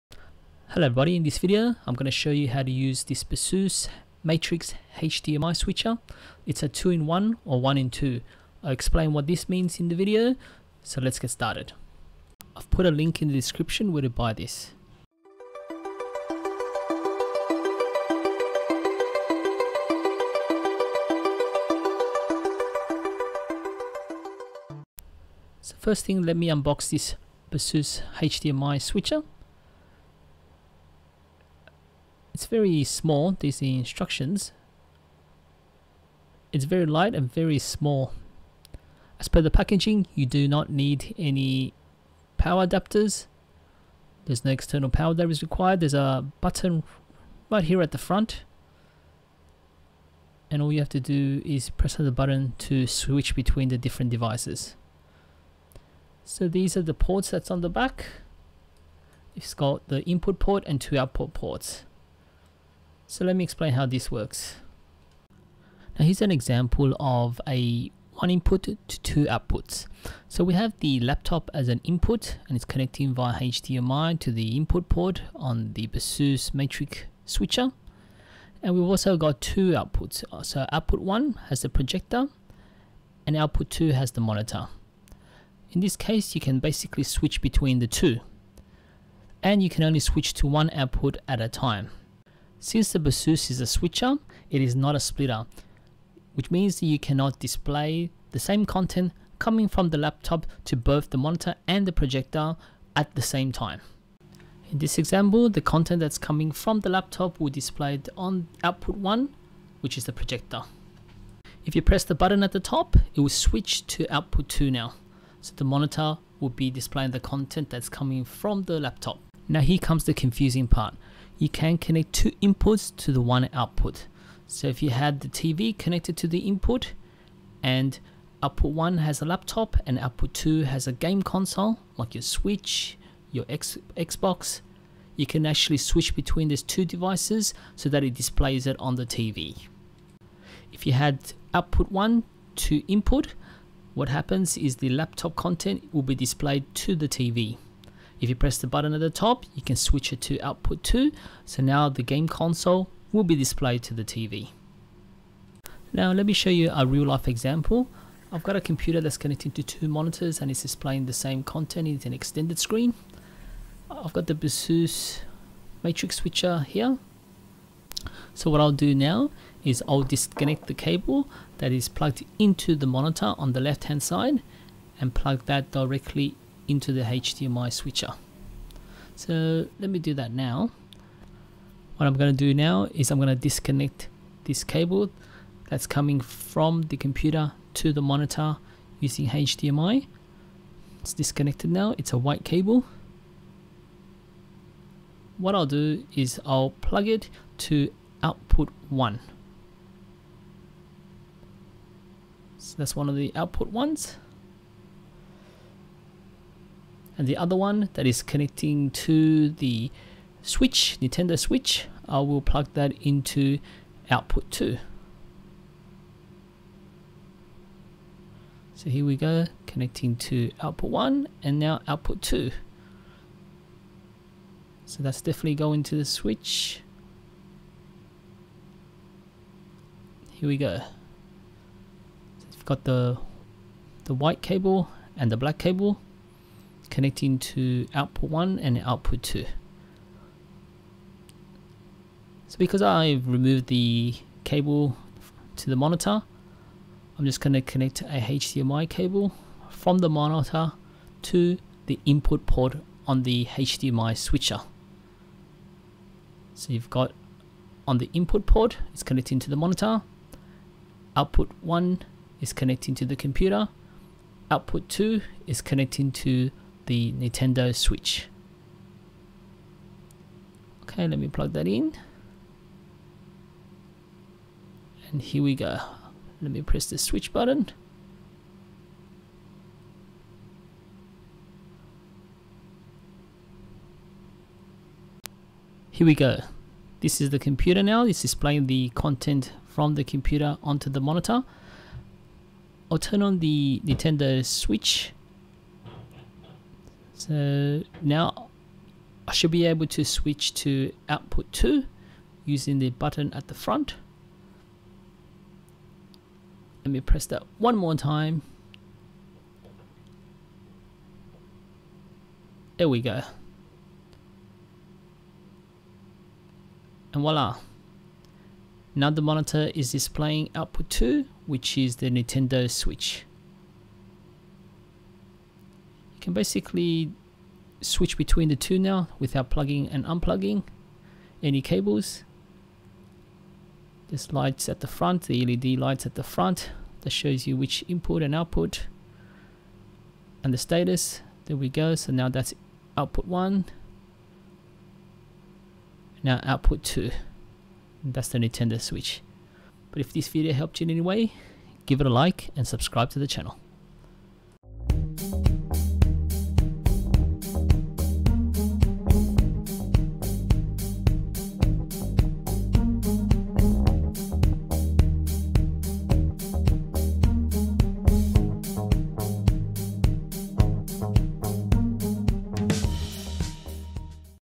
HOW TO USE THE BASEUS MATRIX HDMI SWITCHER
Read this article In this video, I will unbox the Baseus Matrix HDMI Switcher, explain how it works and show you an example of switching between two devices.
The Baseus HDMI Switcher doesn't require an external power source. It can be used as 2-in-1 or 1-in2 (2 inputs to 1 output or 1 input to 2 outputs).
This is perfect if you want to use the same monitor or TV and switch between a gaming console or laptop/computer without having to unplug cables. Supports 4K and Audio.

LINKS:

Voiceover Microphone
Baseus Matrix HDMI Switcher

OUTLINE:
00:00 - Intro
00:10 - Unboxing Baseus Matrix HDMI Switcher
01:05 - 1 Input to 2 Outputs
01:32 - Not a splitter to display the same content
01:47 - Input to output 1
01:51 - Input to output 2
01:59 - 2 Inputs to 1 output
02:20 - Output 1 to input
02:28 - Output 2 to input
02:37 - Real-life example 2 inputs to 1 output (Nintendo Switch and computer to monitor)